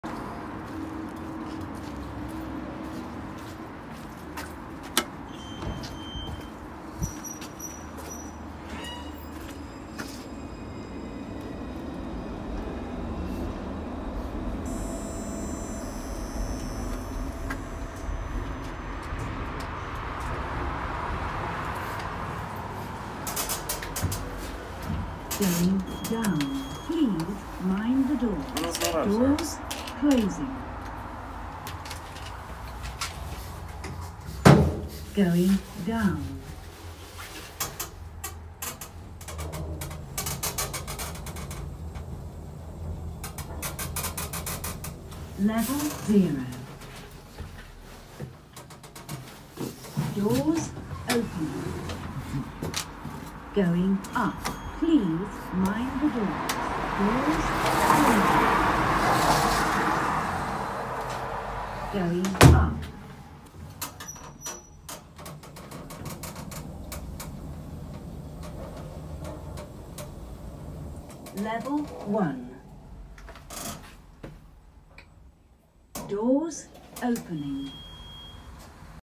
Schindler replaced by Mid Western lift at Charlemont Luas station
Replaced in 2022
Video filmed 10th December 2022